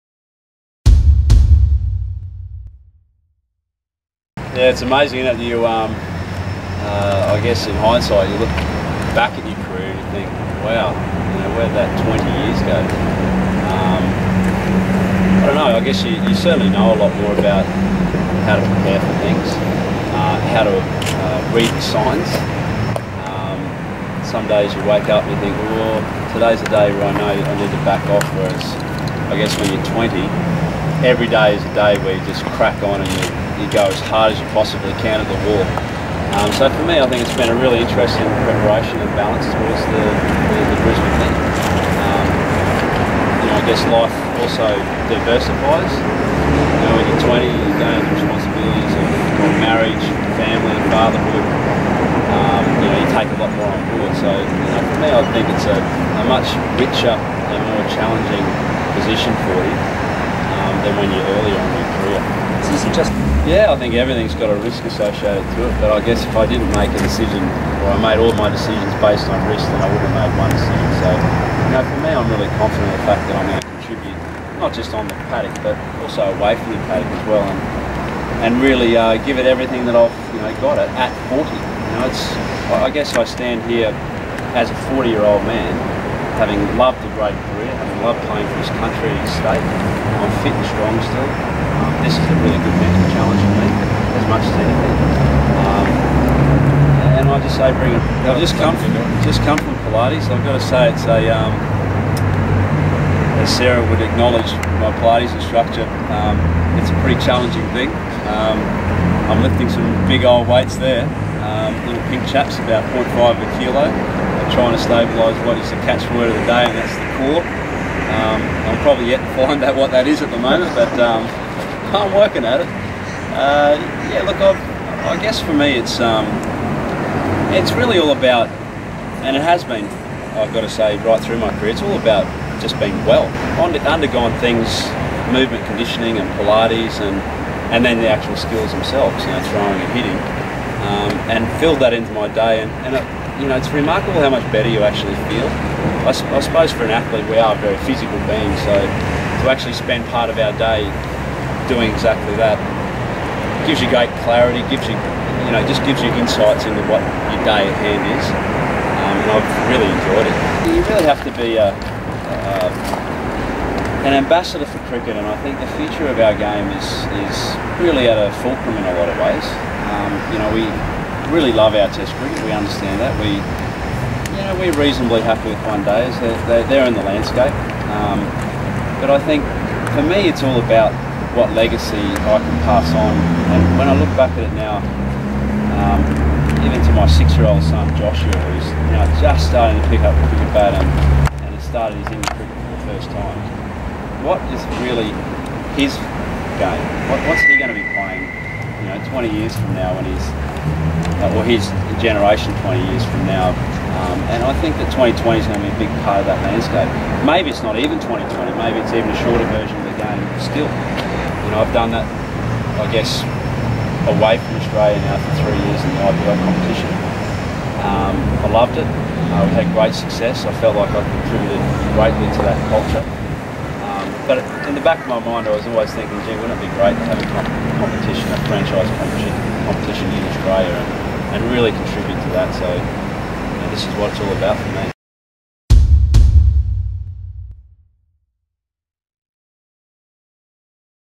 Matthew Hayden Press Conference
Matthew Hayden provides Heaters with an update on his training progress on the eve of his 40th birthday.